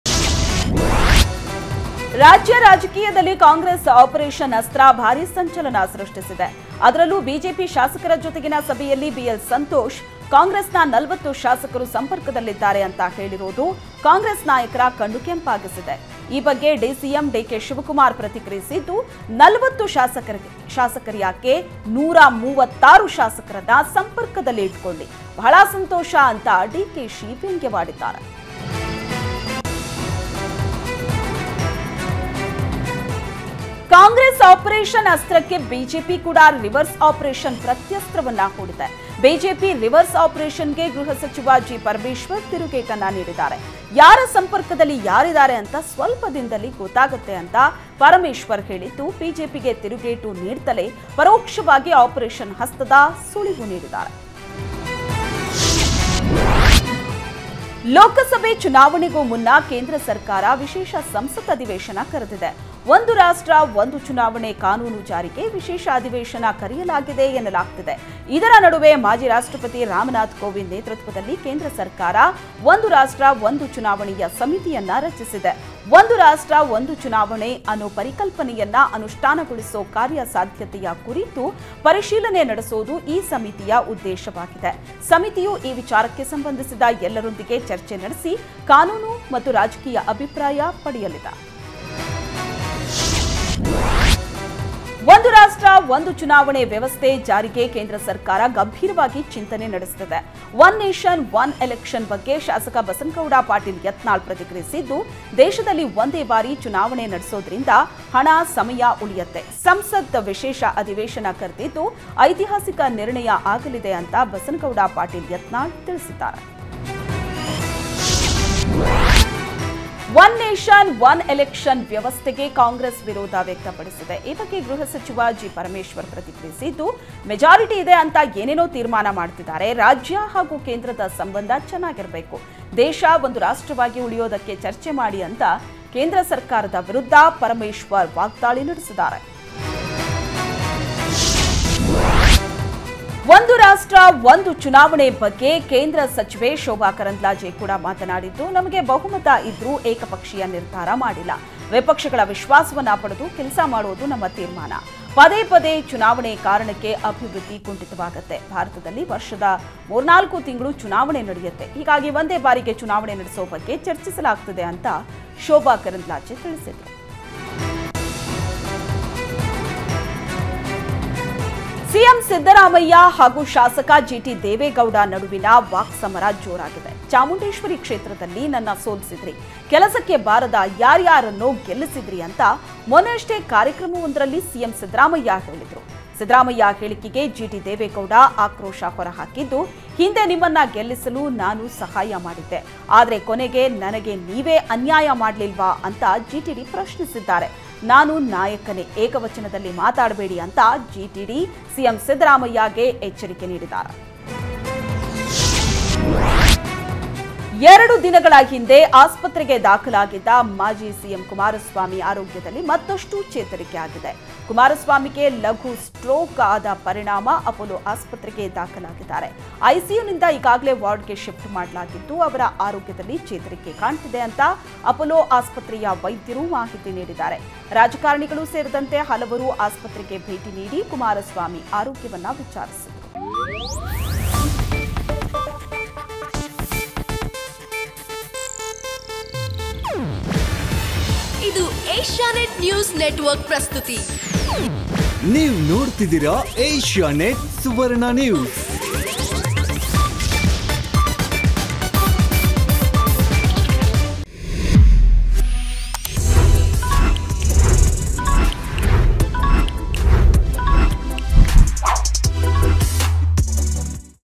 Yatnal : ಒಂದೇ ಬಾರಿ ಎಲೆಕ್ಷನ್ ನಿಂದ ಸಮಯ ಉಳಿತಾಯ | Kannada News | Karnataka News Express - Part 2
ಒಂದು ರಾಷ್ಟ್ರ ಒಂದು ಚುನಾವಣೆ ಜಾರಿಗೆ ಸಿದ್ಧತೆ  | Kannada News |  Karnataka News Express - Part 2